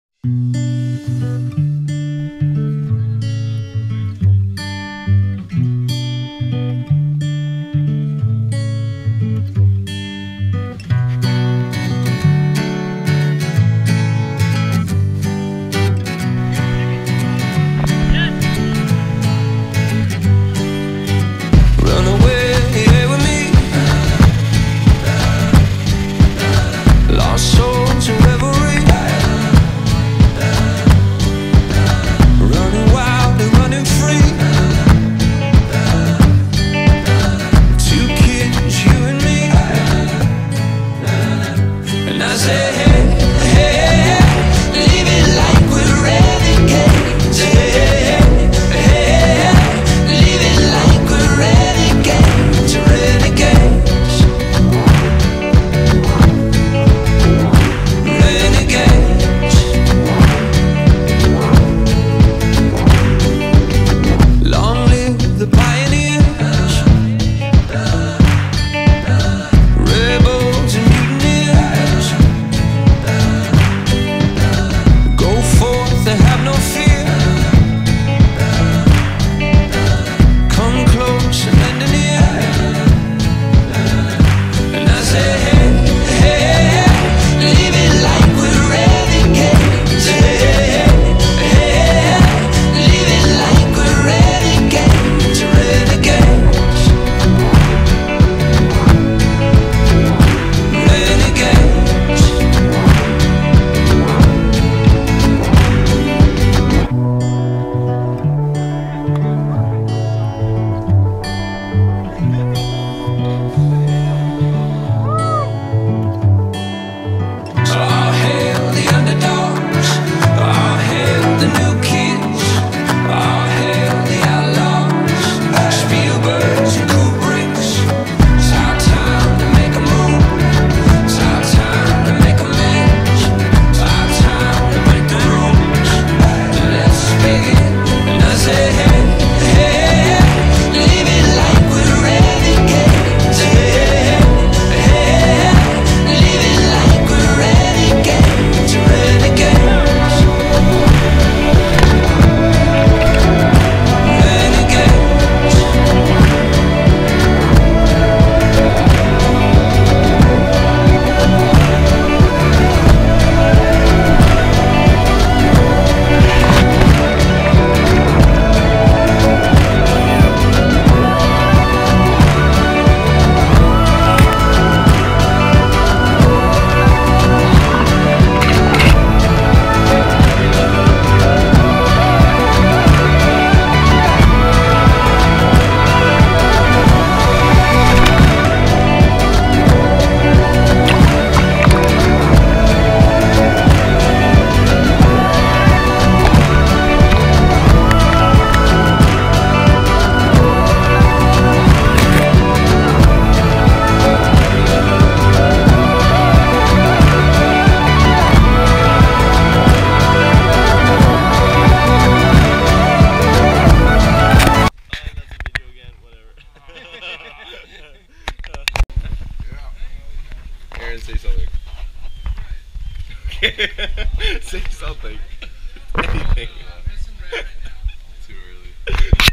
Chris and Brittany's Epic Wedding Weekend (GoPro)
Cliff Jumping Great Falls maryland (MD), and jumping off a dam! I think the jump was 40-50 feet. The water was super low at this time.